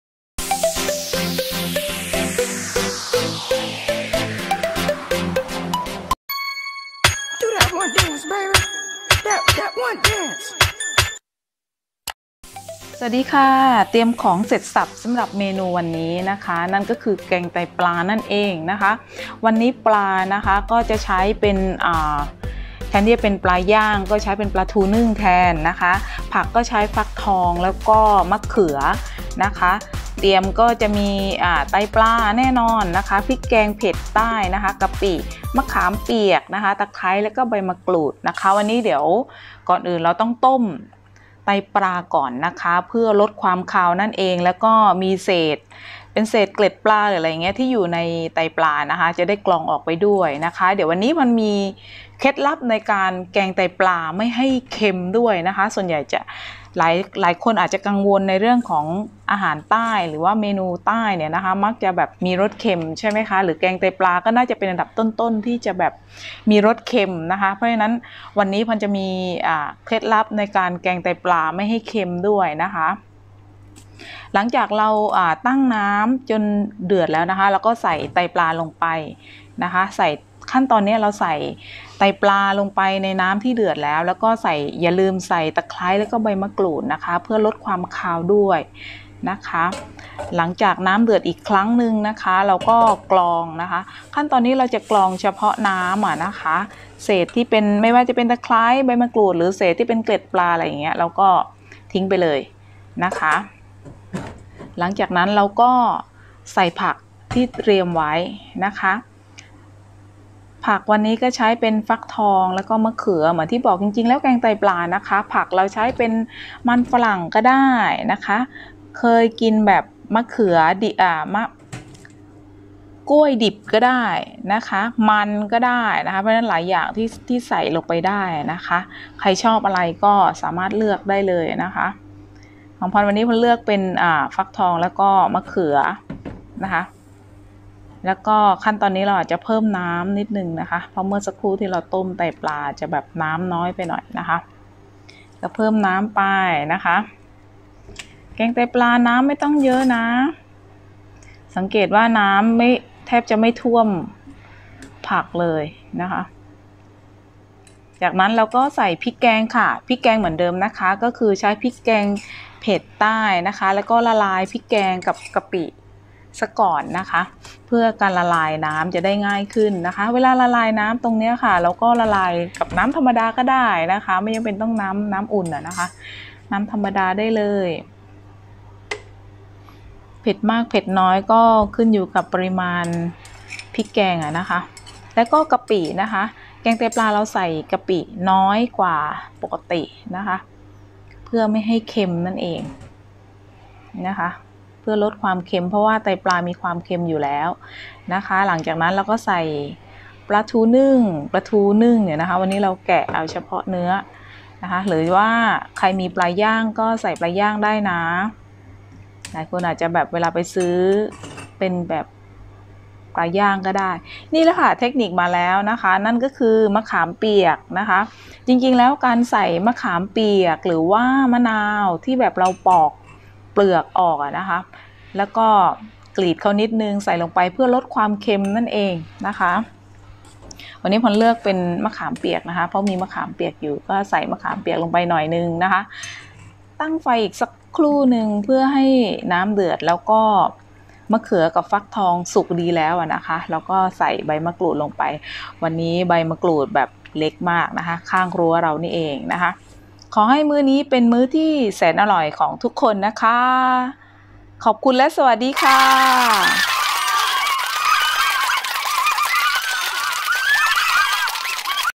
#​ไตปลา เคล็ดลับแกงไตปลาไม่ให้เค็ม | ArponStation​ 4 มีนาคม ค.ศ. 2021
#​แกงไตปลา เป็นอีกเมนู​อาหารใต้ ที่หลายๆท่านอาจจะรู้จักกันอยู่แล้ว วันนี้ ArponStation​ มาบอก "เคล็ดลับแกงไตปลาไม่ให่เค็ม"
ไตปลา
ฟักทอง
มะเขือ
พริกแกงเผ็ด​(ใต้)
กะปิ
ตะไคร้
ใบมะกรูด
มะขามเปียก
#​ไตปลา
#​เคล็ดลับ
#​เมนูอาหารใต้